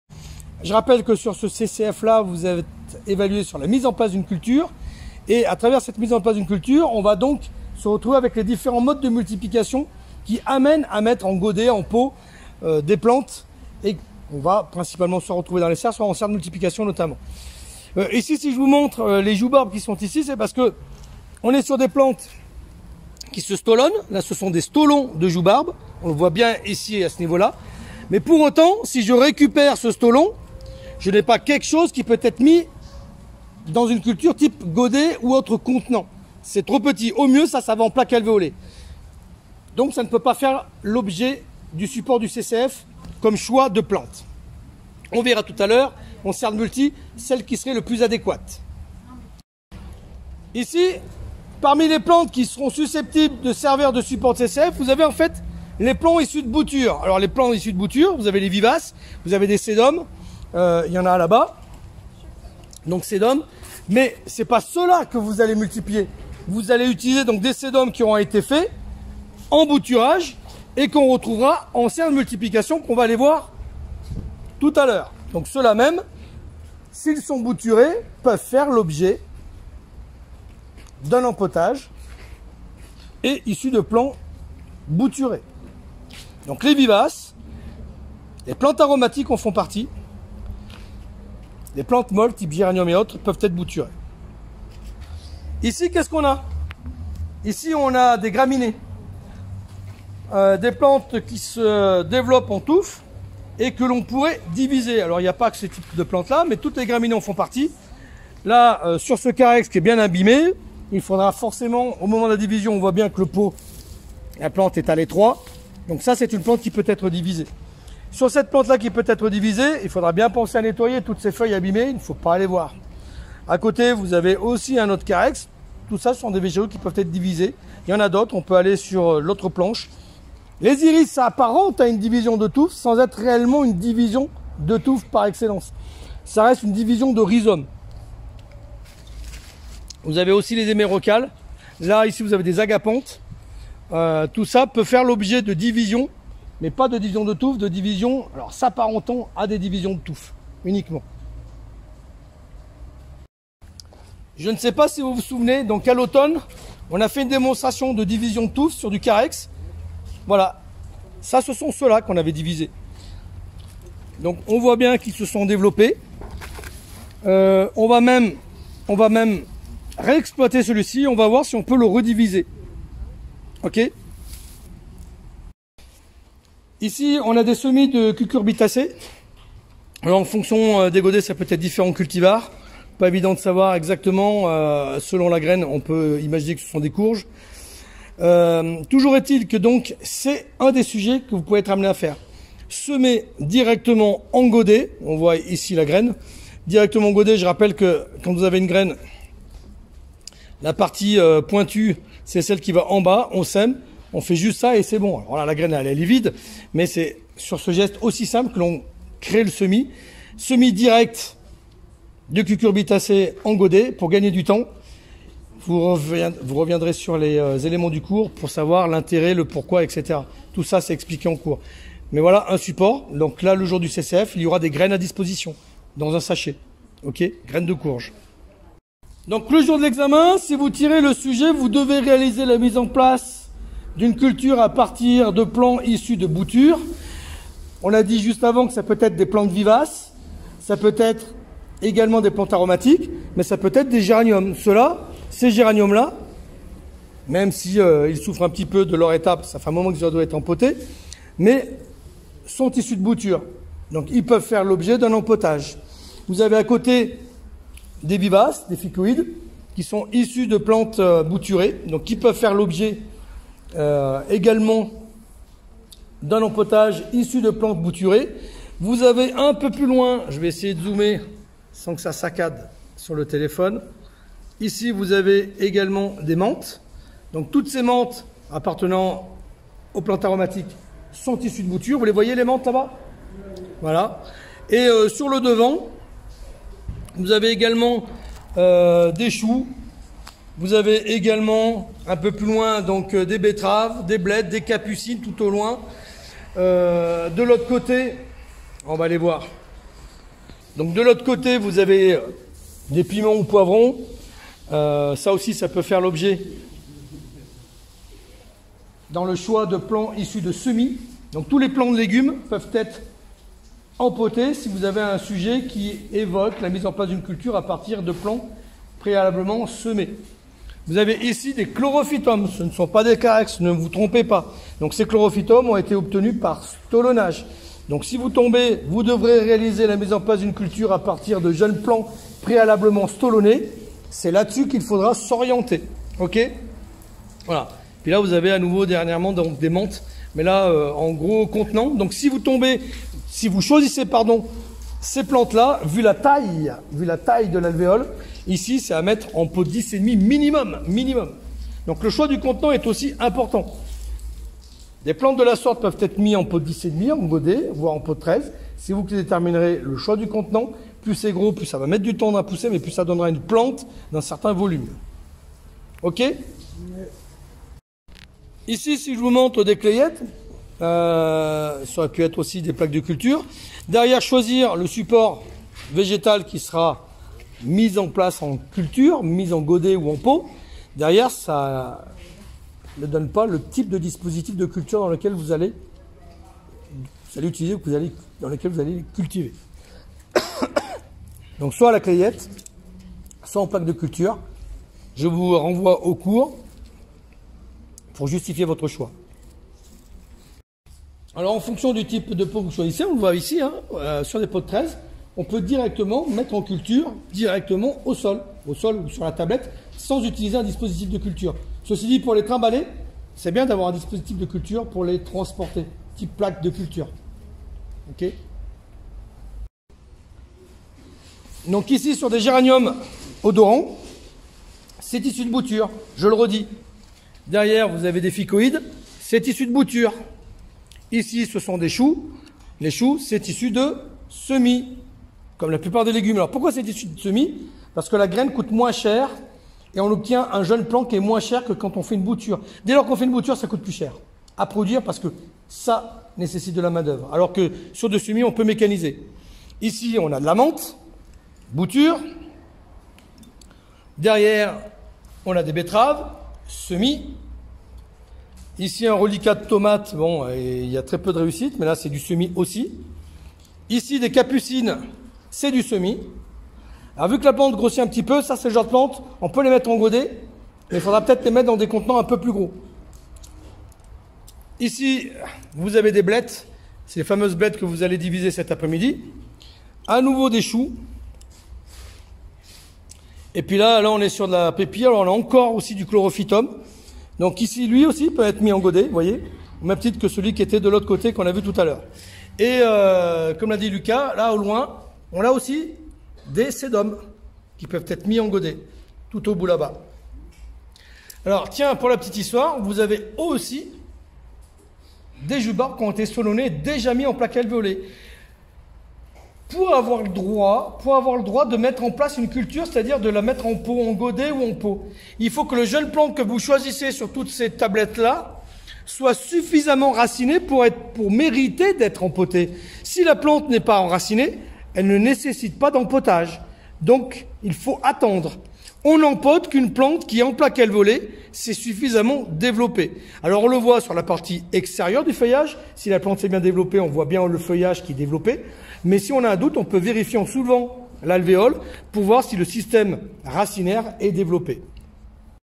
Préparation du ccf E4.2 : mise ne place d'une culture : choix du végétal - mai 2023
Lors du ccf les élèves devront choisir le végétal adapté selon le sujet tiré au sort, parmi des plants stolonnés, semé, bouturé ou à diviser.